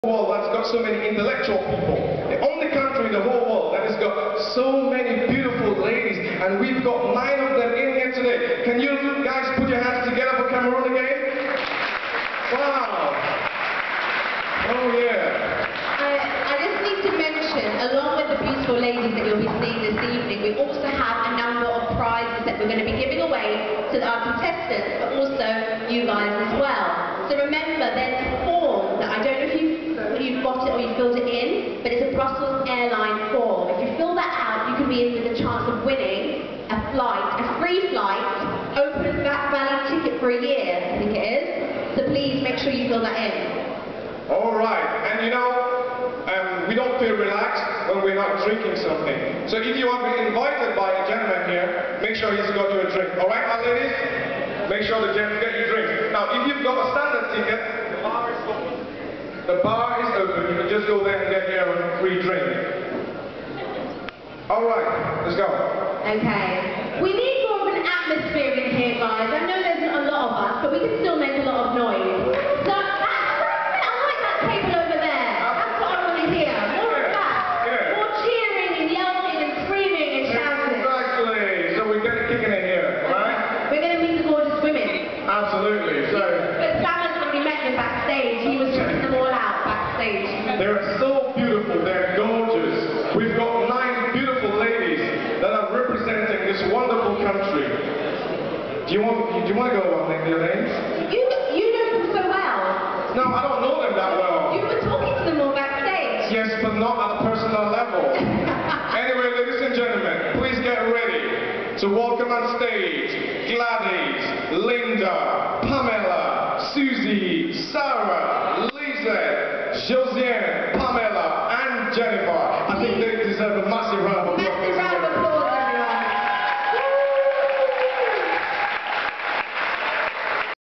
[okabol.com] Sama Ndango & Jordine Bartlett at Miss Cameroon-UK 2011
Sama Ndango & Jordine Bartlett hosted the Miss Cameroon-UK 2011 in London, saturday 26th November. Video by Jean-Pierre ESSO,